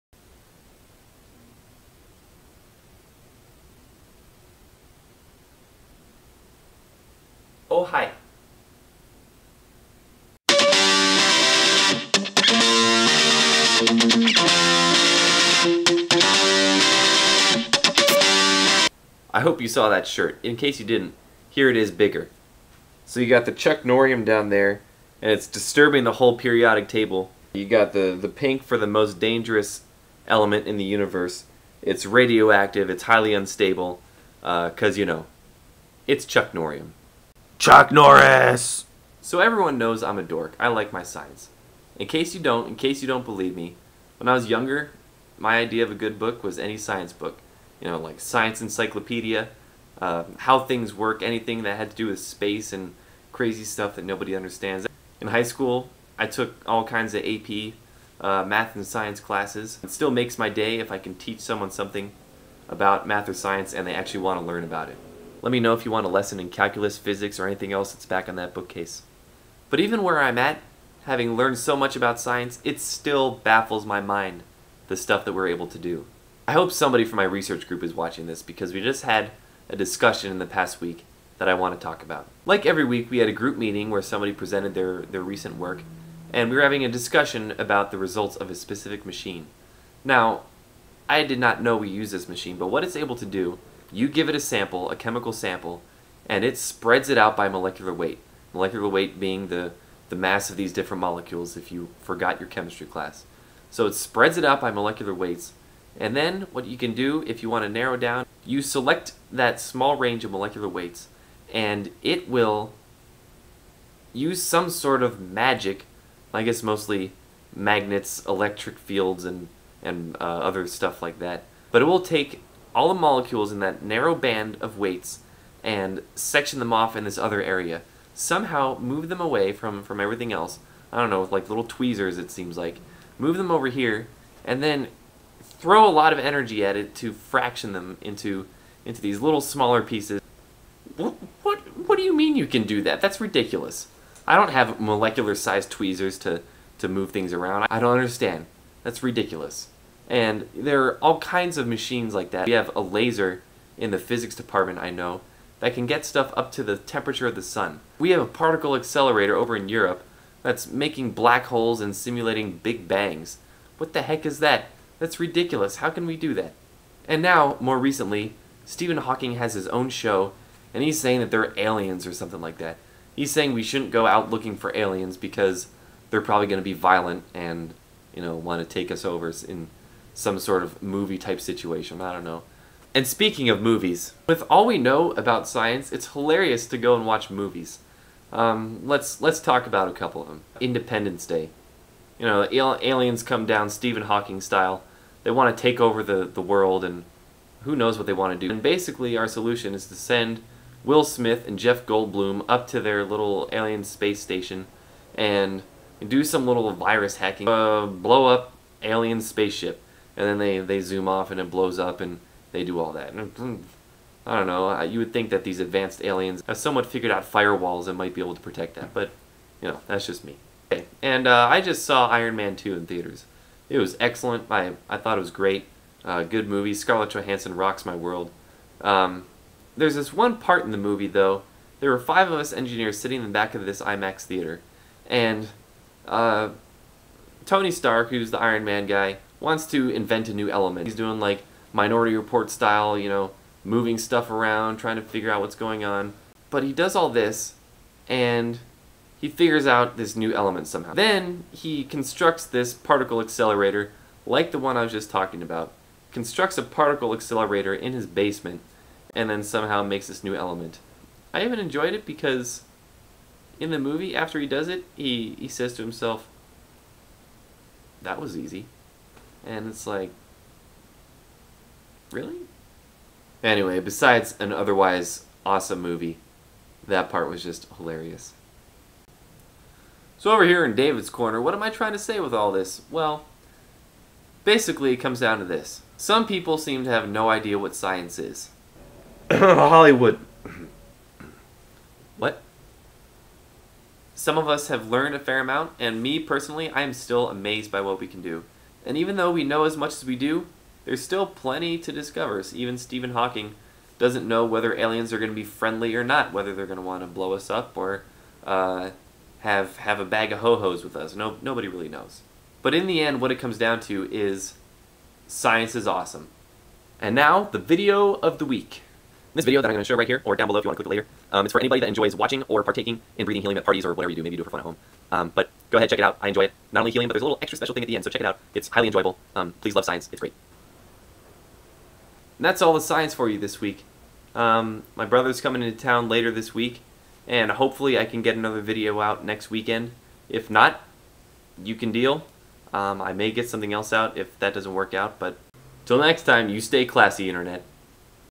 Science is Awesome
I'm an engineer.  There's no arguing that I have a certain fondness for science.  I got a new shirt that brings out my inner chemical engineer, so I thought I'd show it off.  While I'm at it, here are my thoughts on science in general.

-Helium makes your voice sound really high, but can anything make it go deeper?